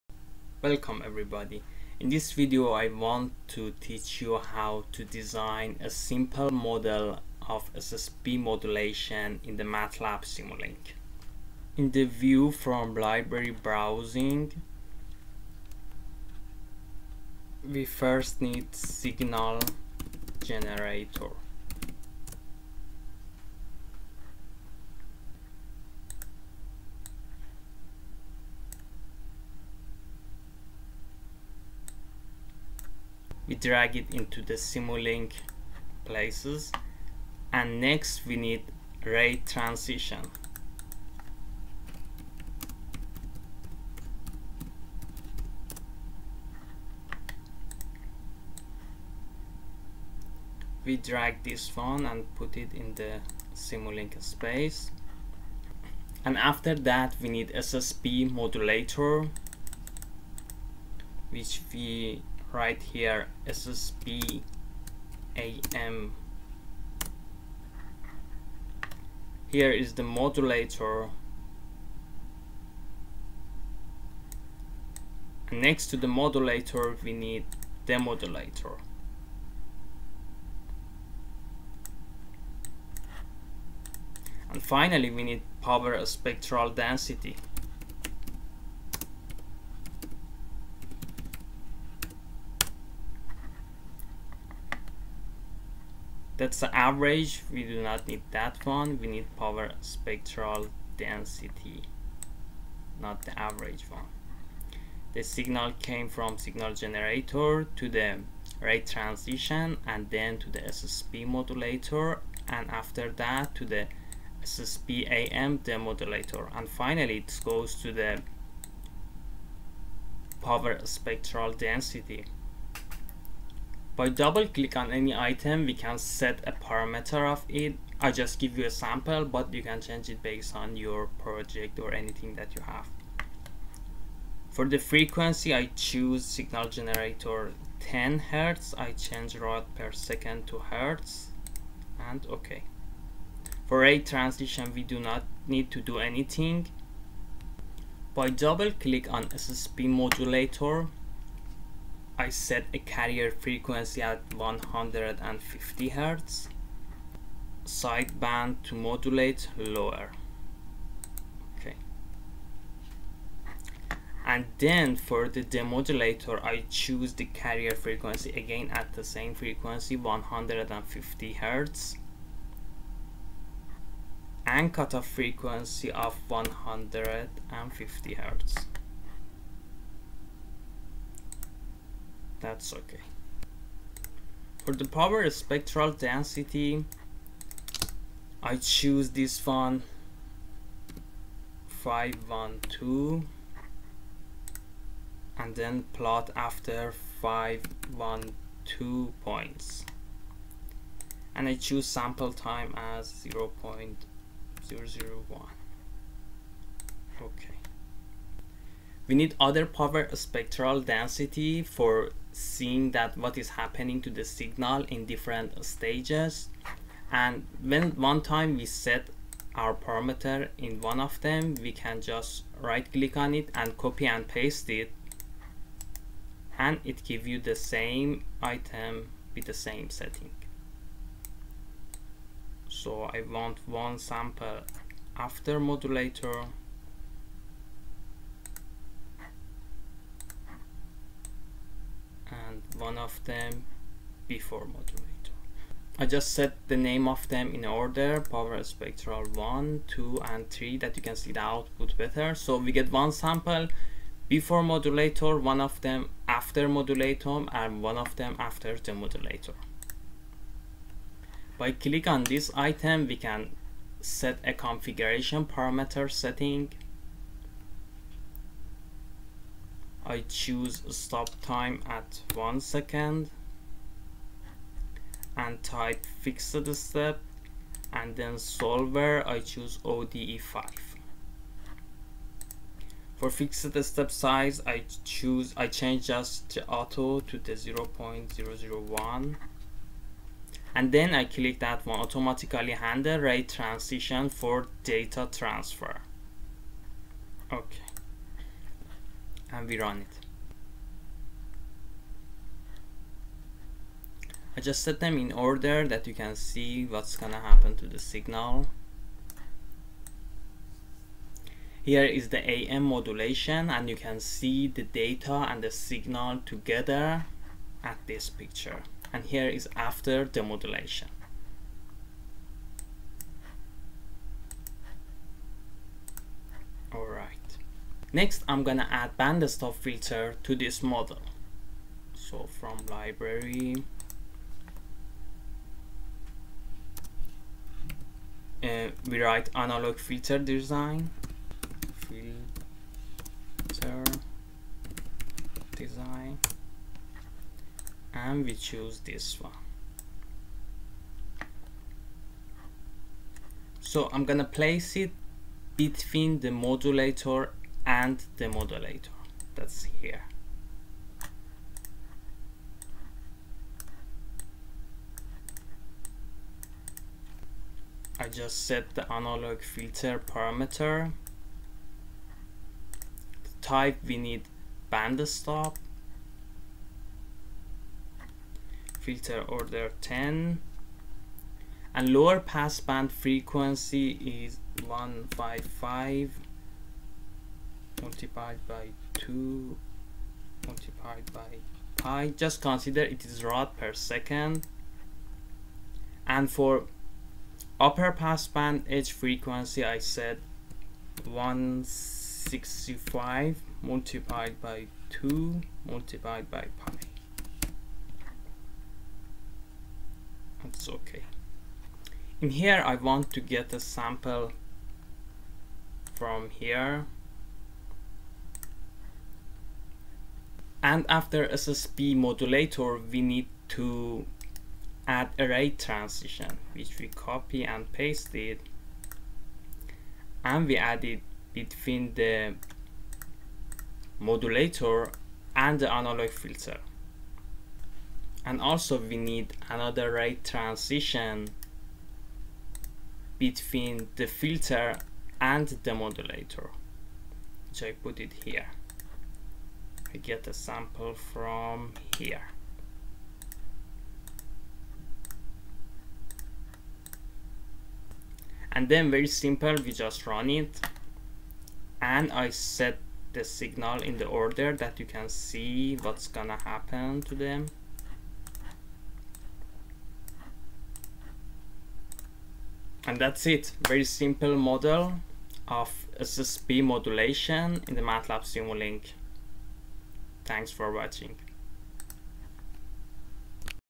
SSB Modulation Matlab
Here I show you how to simulate a simple SSB model in Matlab Simulink.
SSB is an abbreviation for Single sideband.
Next, I use band-stop filter in the model.
The band-stop filter is available in the DSB Blockset/Filter Designs library (Analog Filter Design).
It is applicable especially at the Communications Systems lab in the section of Amplitude Modulation.
Please make comment on what do you want to learn next in the area of telecommunications.